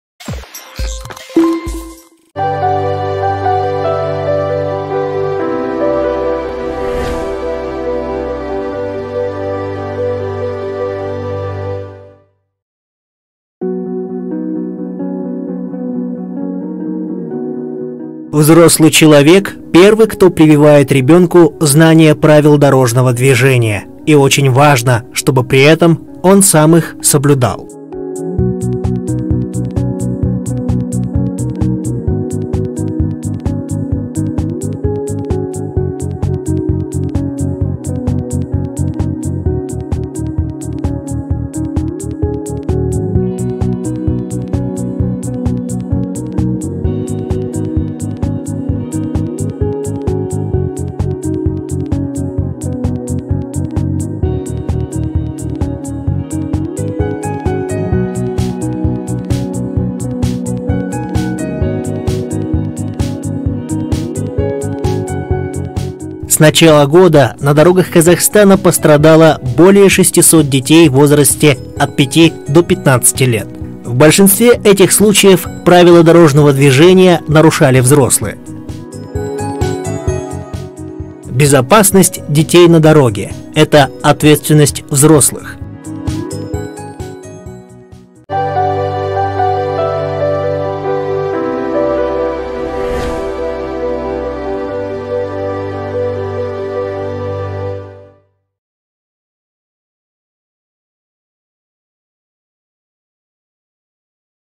Безопасность на дороге
Эта серия видео от Комитета по охране прав детей МОН РК содержит актуальную информацию для детей и родителей по актуальным социальным вопросам. Ознакомьтесь с содержанием информационных роликов и записей социальных экспериментов, а также узнавайте важную информацию по защите прав детей. Просим делиться этими видео во всех доступных средствах коммуникации, и надеемся, что это станет реальной помощью в важных социальных вопросах.